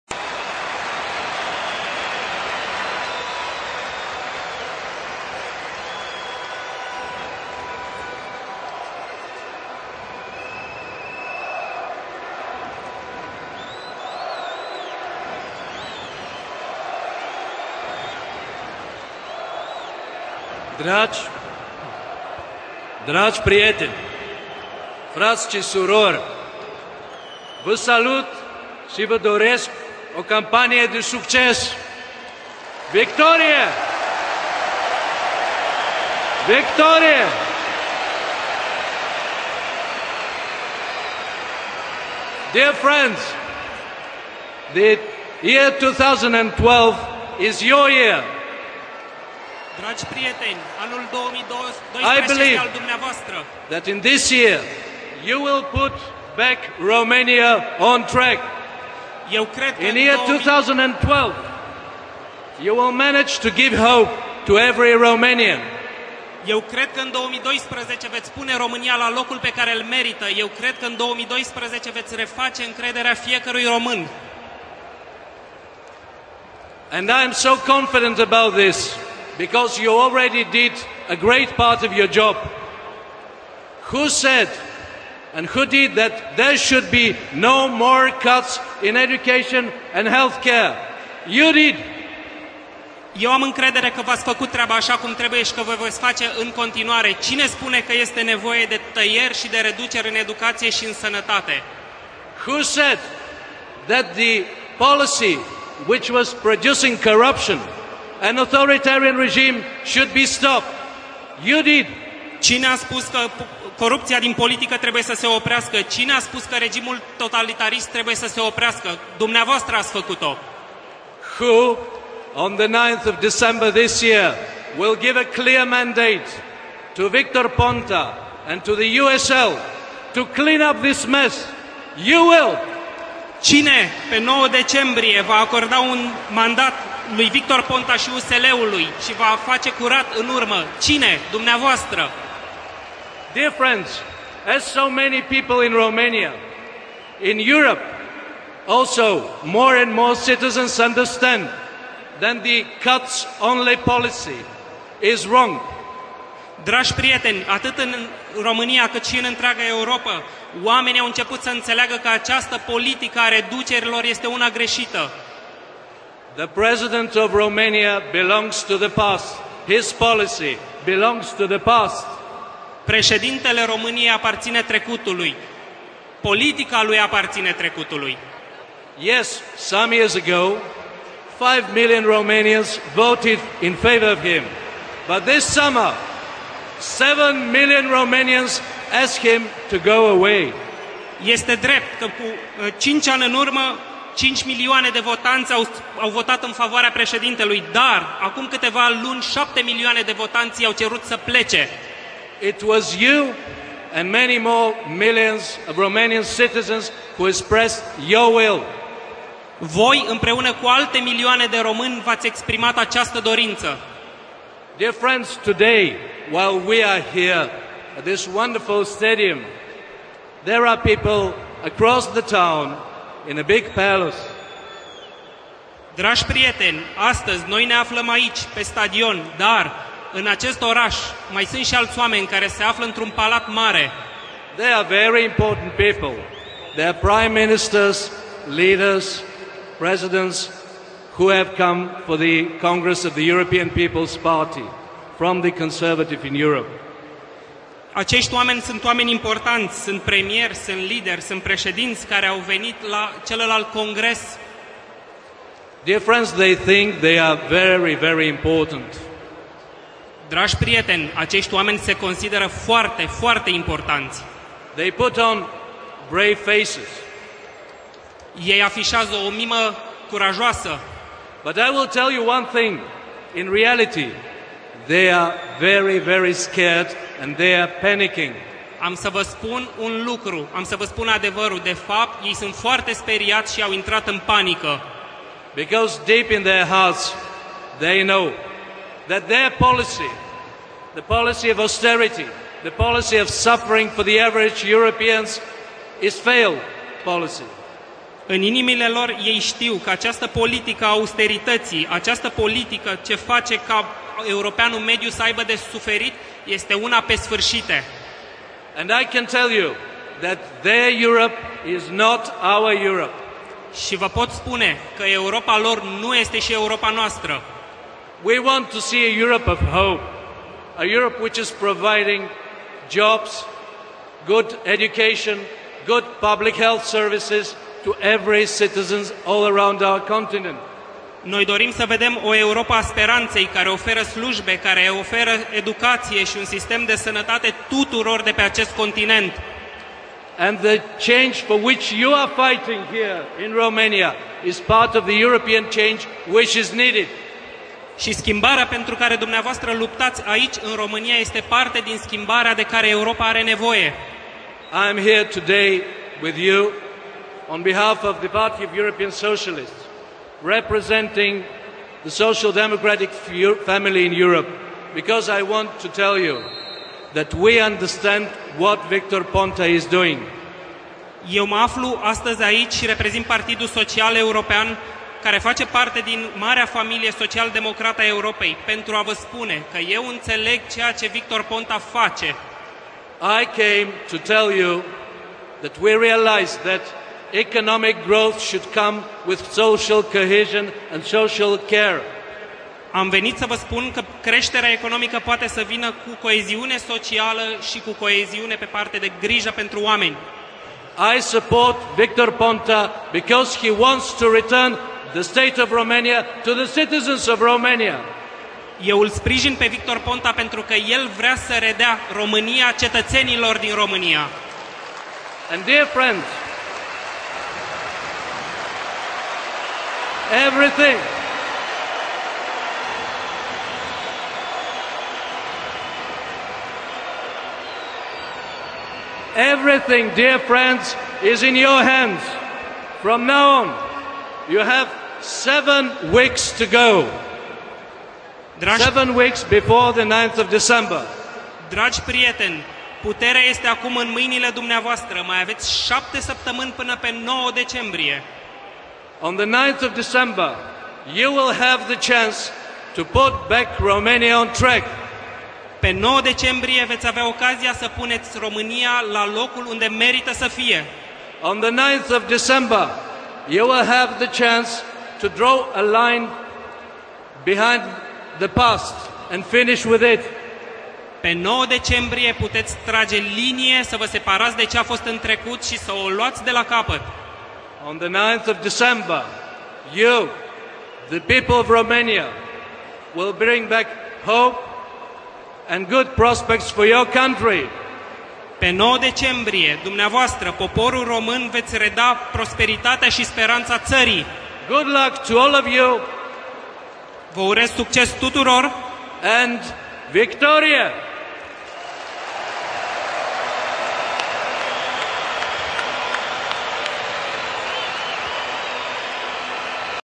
Stanishev speaks to 80,000 at Bucharest USL election rally
On Wednesday, 18 October, Party of European Socialists (PES) President, Sergei Stanishev, attended the launch of the USL election campaign. Romania will go to the polls on 9 December. He addressed 80,000 supporters of the coalition.